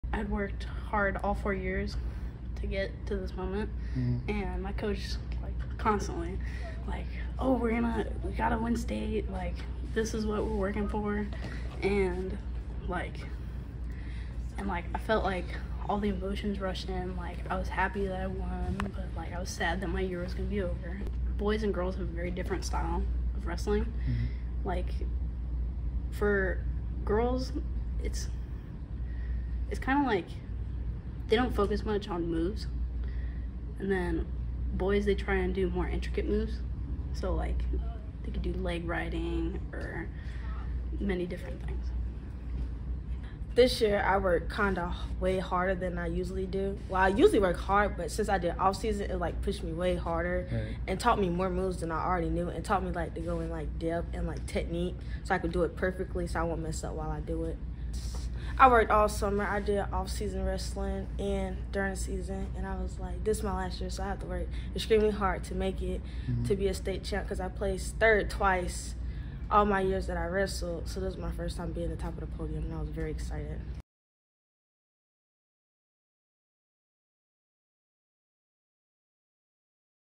Rock Hill Girls Wrestling State Champions
Ta’Niya Roberts and Kalijah Campos are athletes on the Rock Hill High School girls wrestling team that won the state championship.

Video by Tracy Kimball/The Herald